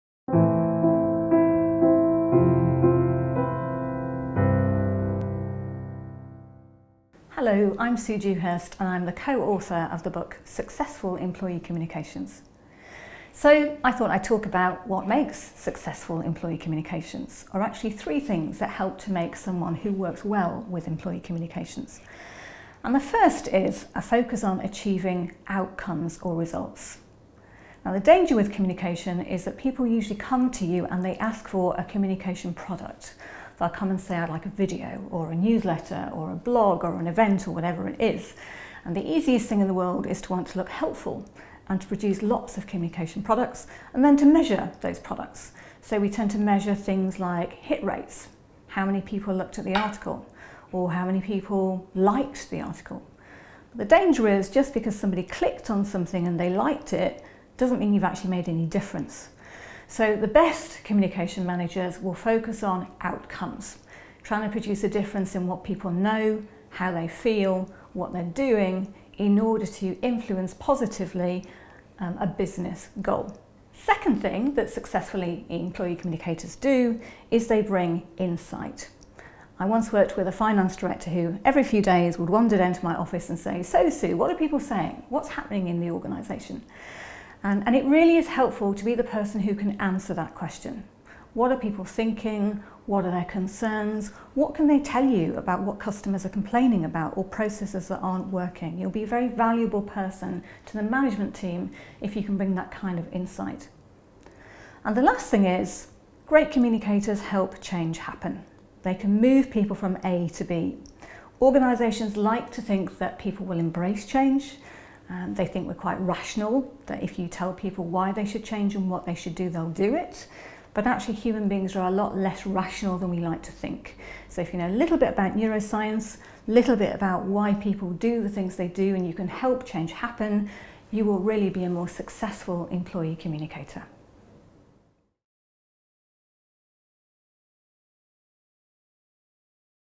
Improve Your Employee Communications | Sue Dewhurst
Make your employee communications effective by employing three key techniques that will ensure change happens through focused results and outcomes.